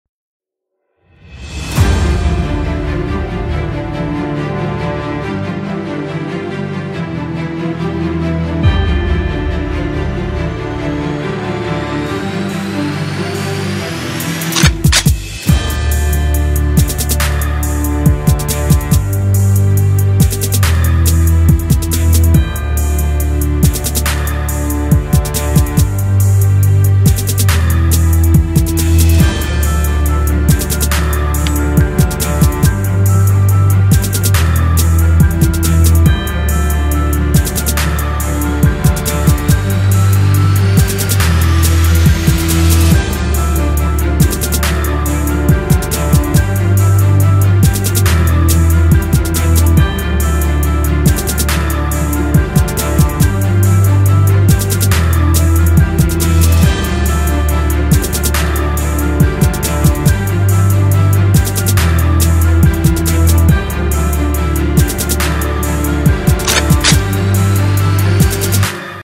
HAND-PAINTED AK-74 SCULPT SHOWCASE (NOMADSCULPT)
Hi, i made a stylized hand-painted sculpt of Ak-74 in Nomadsculpt - Just for test.